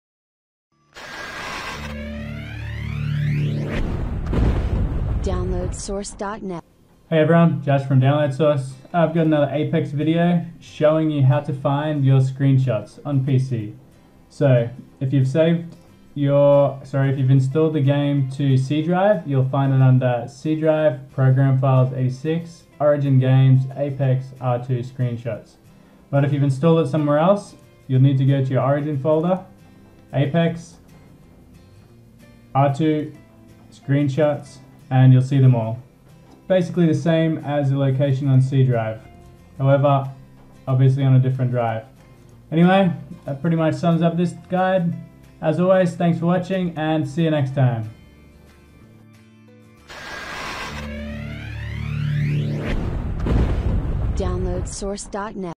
Where Are Apex Legends Screenshots Kept? (Apex Legends Screenshot Folder Location)
A short guide showing you where to find screenshots taken in Apex Legends on PC.